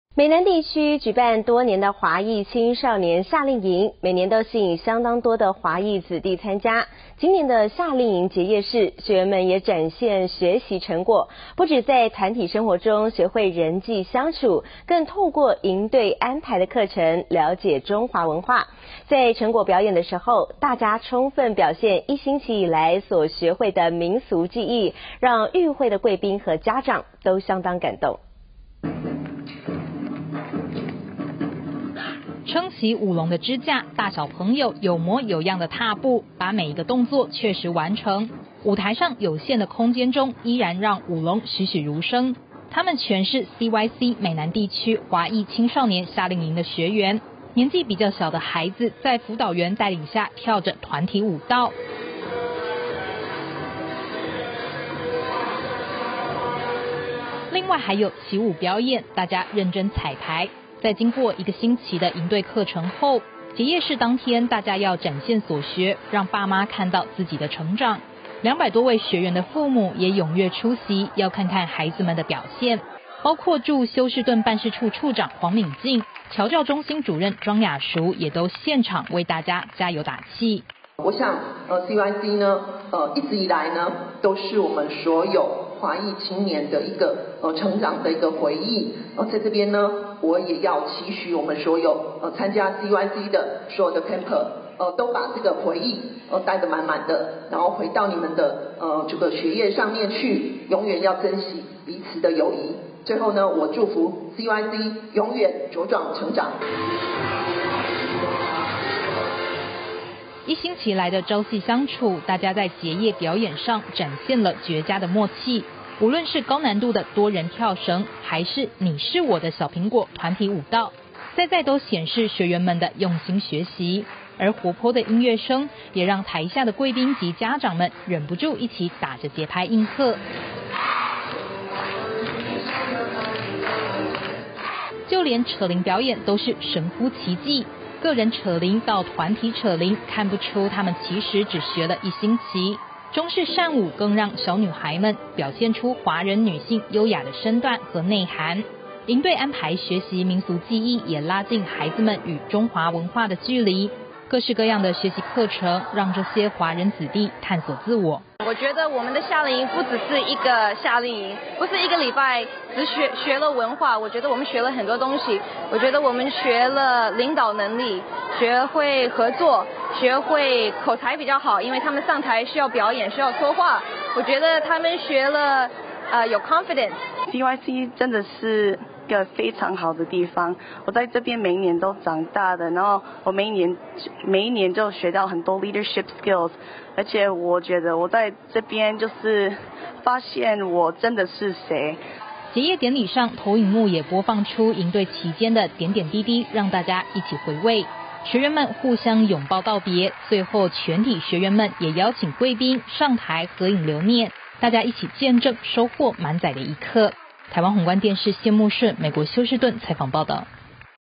CYC華裔青少年夏令營結業成果展—Overseas Compatriot News 宏觀僑社新聞
美南地區舉辦多年的，華裔青少年夏令營，每年都吸引相當多華裔子弟參加。今年的夏令營結業式，學員們也展現學習成果，不只在團體生活中，學會人際相處，更透過營隊安排的課程，了解中華文化。在成果表演的時候，大家充分表現一星期以來，所學會的民俗技藝，讓與會貴賓和家長，都相當感動。

臺灣宏觀電視 謝慕舜 (編輯 陳瀲文)
美國休士頓 採訪報導